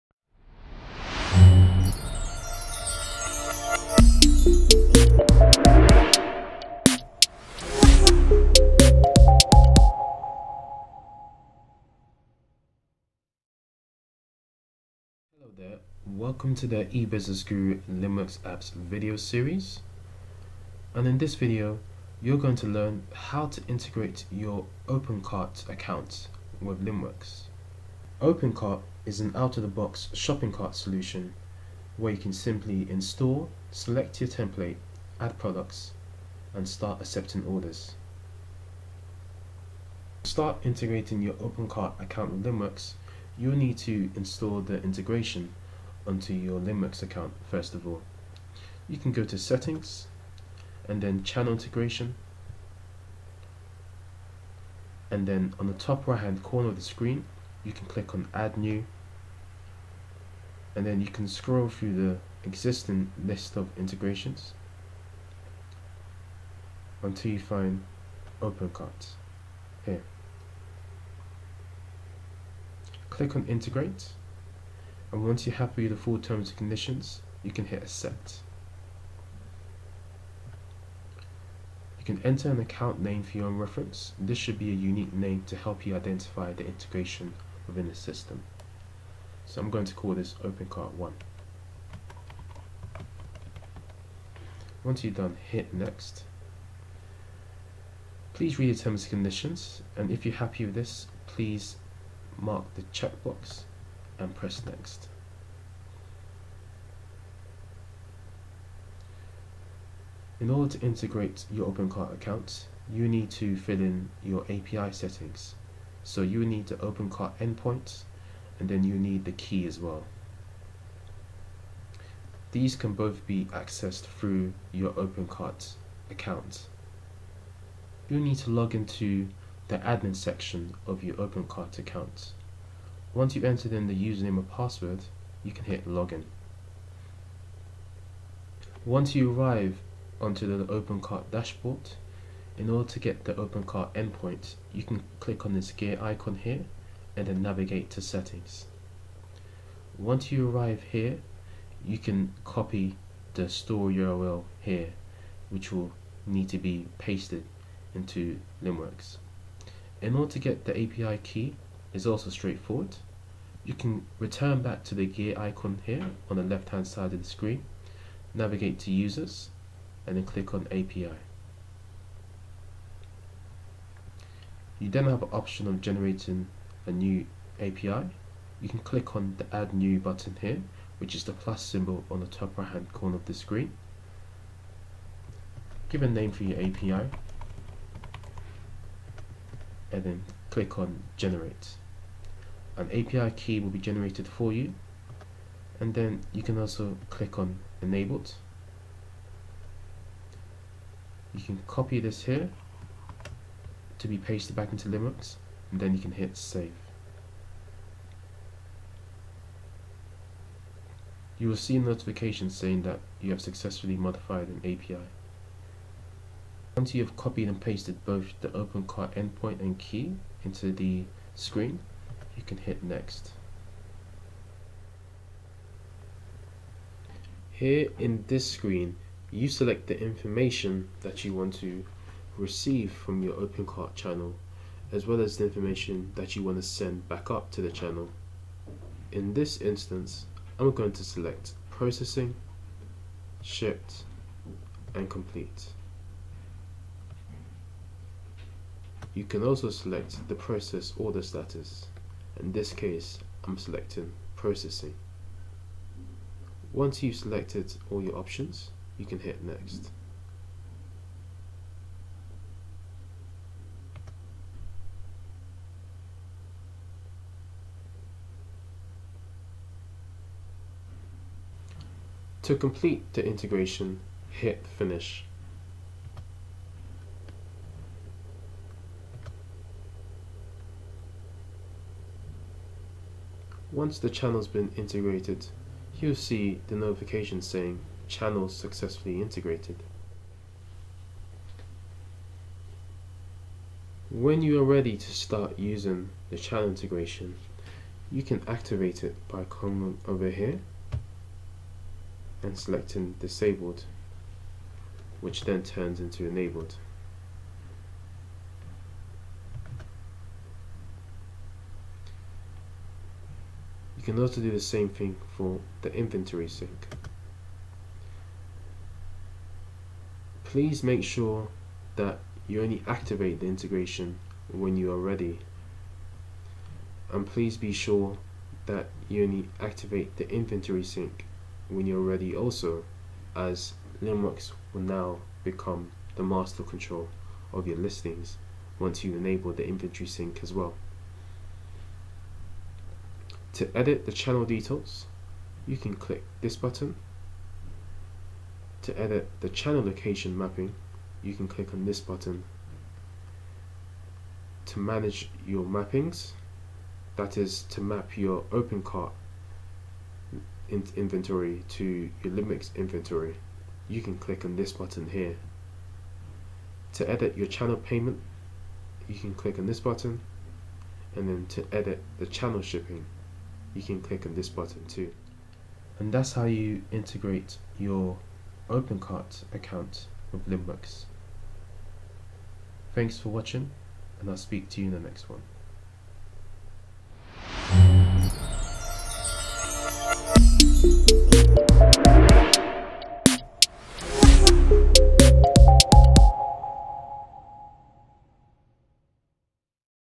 How to Integrate your OpenCart account with Linnworks
OpenCart is a free and open source e-commerce solution which can be used to create e-commerce websites. This has been around since the late 90s and still finds itself to be a popular e-commerce template solution. It has a range of themes, modules, integrations, and add-ons, that users can use to customise their e-commerce website built on this platform.

Monthly packages are £24.99 + VAT/month.

Ebusiness Guru is an eCommerce agency specialising in Ecommerce services, web design, and digital marketing.